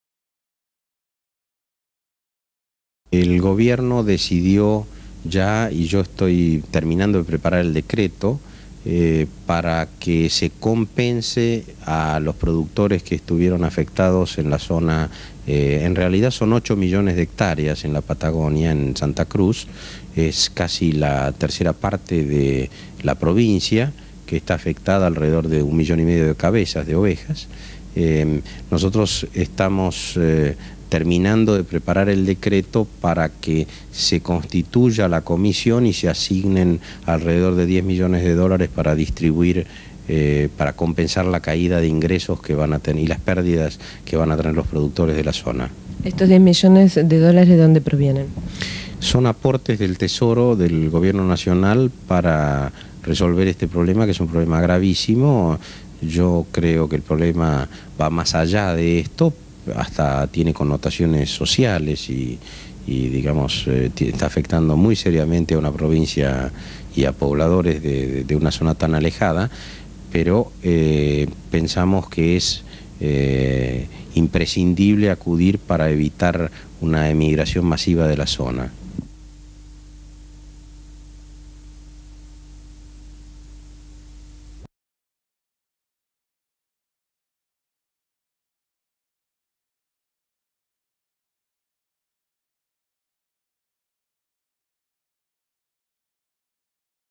DiFilm - Compensacion a productores afectados del volcan Hudson 1991
Bs. As.: Reportaje a un funcionario donde habla de la compensación a los productores que estuvieron afectados en la Patagonia, a raíz del desastre ecologico con las cenizas del volcan Hudson provocando la mortandad de ganado ovino. Insert ovejas muertas en Santa Cruz y cenizas volcánicas esparcidas por en el terreno.
Fecha: 17/9/1991
Duración: 1 minuto 21 segundos
Código: UG-2793